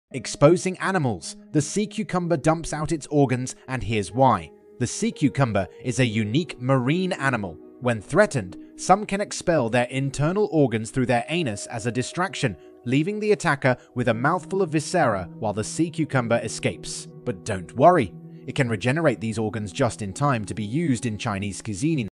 Exposing Animals: THIS SEA CREATURE DUMPS OUT IT'S ORGANS!?
Today we learn about the Sea Cucumber and its unique ability to defecate its organs.